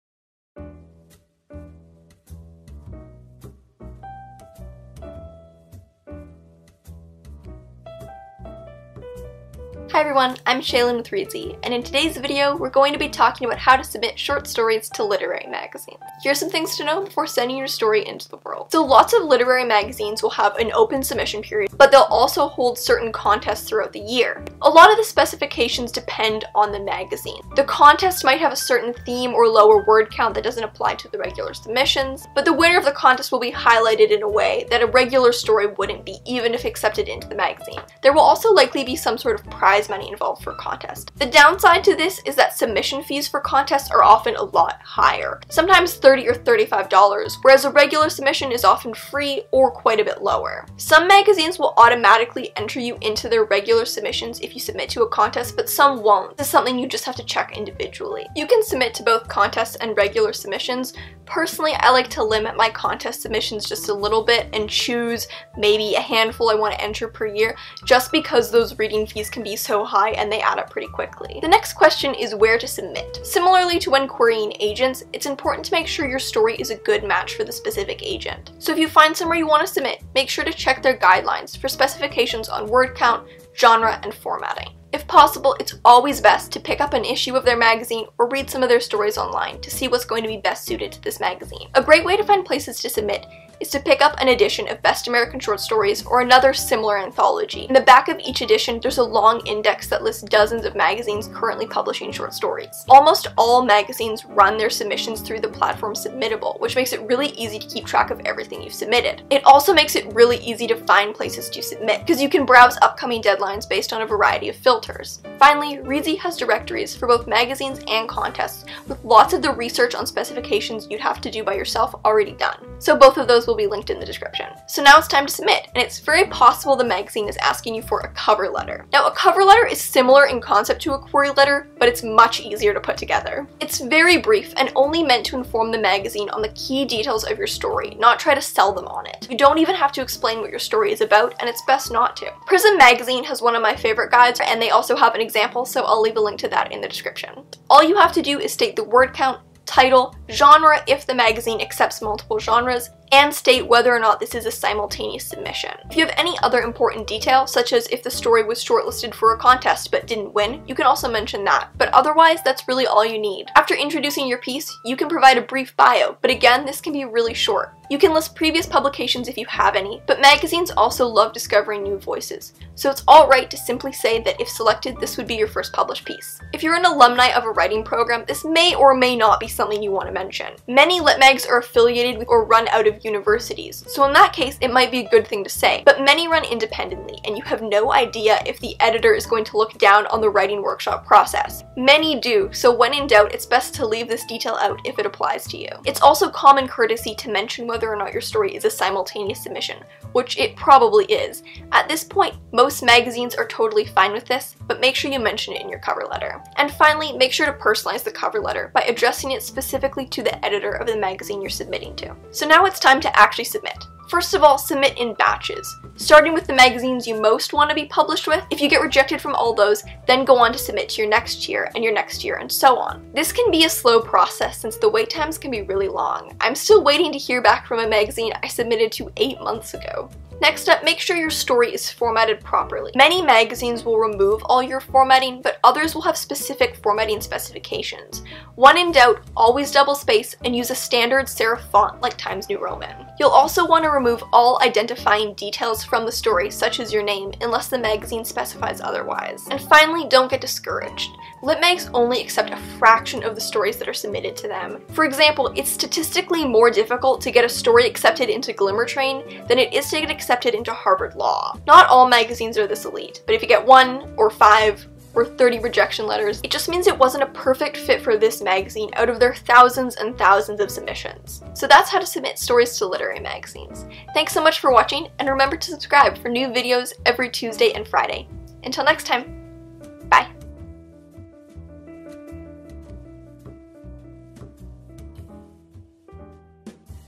How to Submit Short Stories to Literary Magazines
So you've written a short story and...what do you do with it now? Today, we're taking about the short story submission process: how to find places to submit, how to write a cover later, and how to submit strategically.

TIMESTAMPS:
0:00 - Intro
0:18 - Open submissions vs. Contests
1:11 - Where to submit
2:05 - Cover letters
3:32 - How to submit